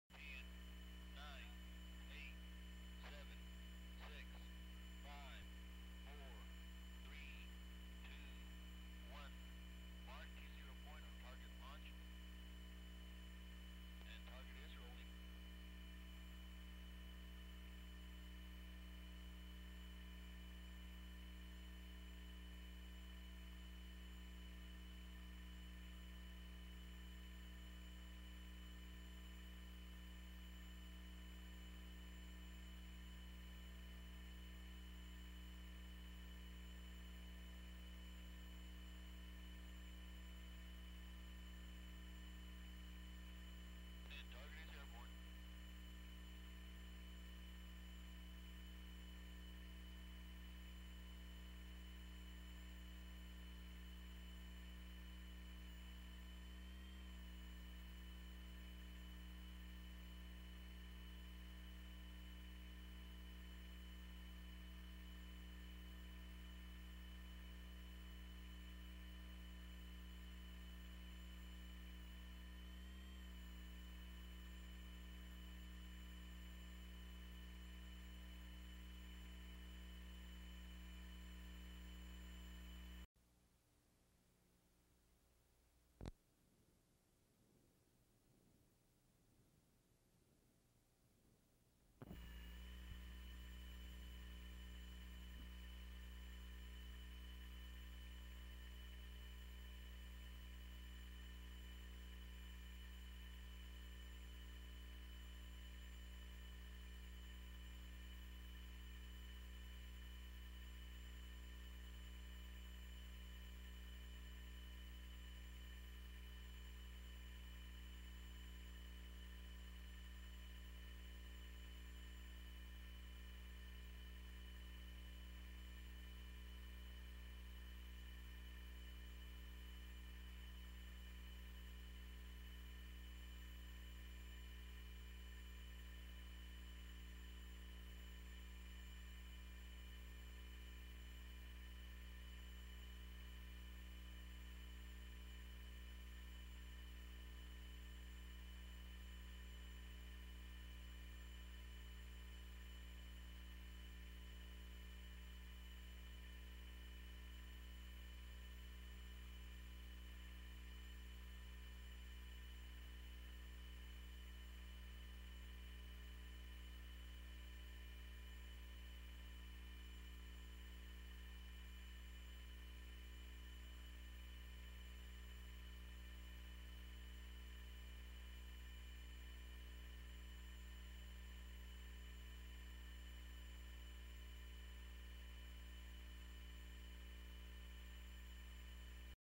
QF-106 Delta Dart Drone Takeoff and Landing at Holloman AFB
An F-106 Delta Dagger converted into an unmanned supersonic drone takes off and lands at Holloman Air Force Base in New Mexico on 02 June 1989. The QF-106 drone was used in the decades after the Vietnam War to test anti-aircraft systems, to include Stinger and Patriot.

Edit: The original archival records and descriptions for this video misidentified the drone as an F-102 Delta Dagger. Thanks to @LV_FUD80 and @bengardiner3867 for the correction.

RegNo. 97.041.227